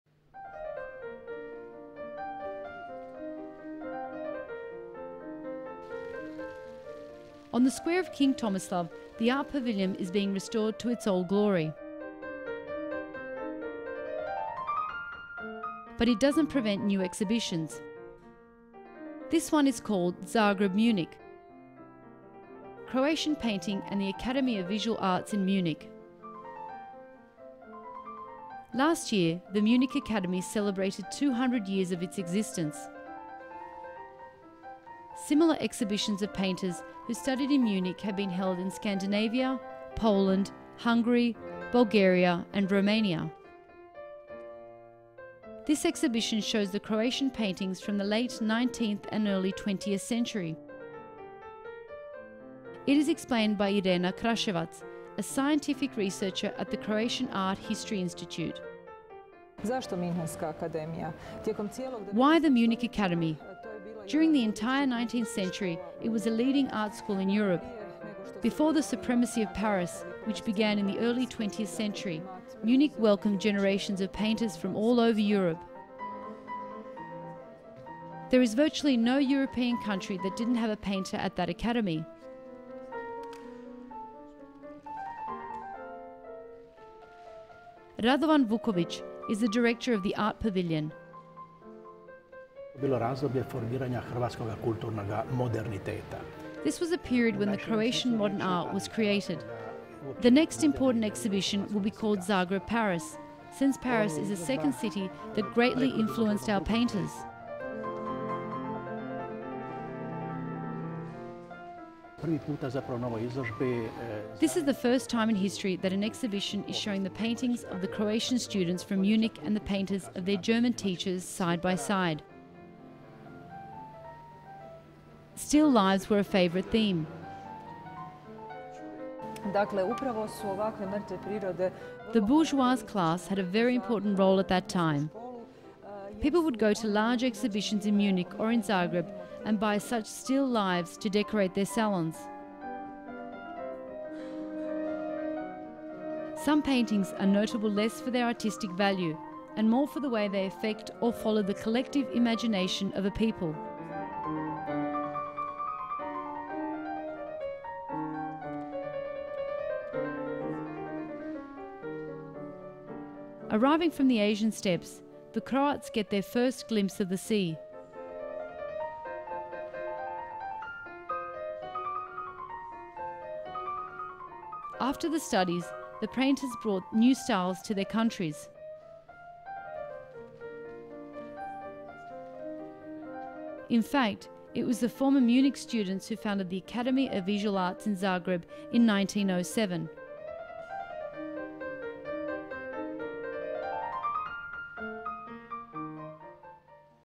The Art Pavilion - "Croatian Painting and the Academy of Visual Arts in Munich"
Marking 200 years of existence of the Munich Academy, Zagreb's Art Pavilion recently exhibited works by Croatian artists, students of this famous German academy.